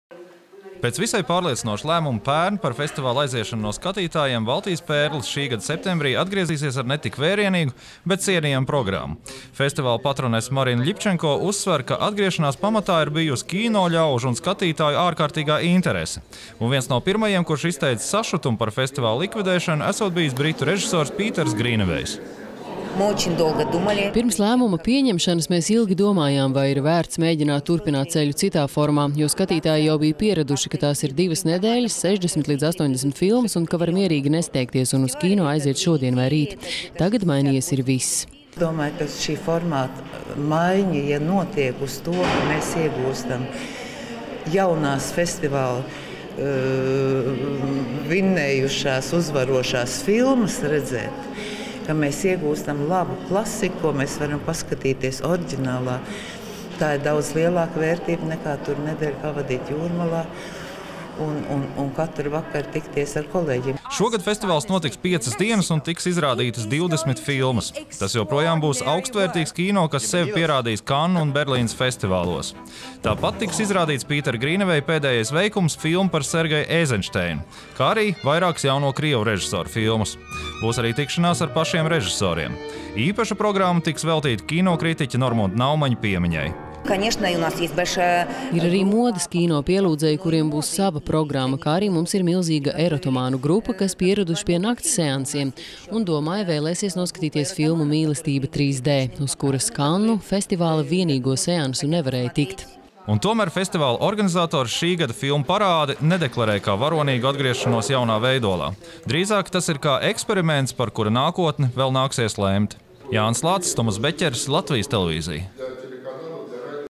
Atgriežas kinofestivāls "Baltijas pērle"
Pirms nepilna gada kinofestivāla “Baltijas pērle” vadība paziņoja par tā slēgšanu, sagādājot lielu vilšanos kinomīļiem, kurus jau bija sarūgtinājusi Baltijas Pērles ilglaicīgā sāncenša - kinoforuma “Arsenāls” izzušana. Tomēr, arī šogad festivāls būs, un notiks no 17. līdz 21. septembrim. Tas gan nebūšot tik vērienīgs kā iepriekšējos 22 gadus.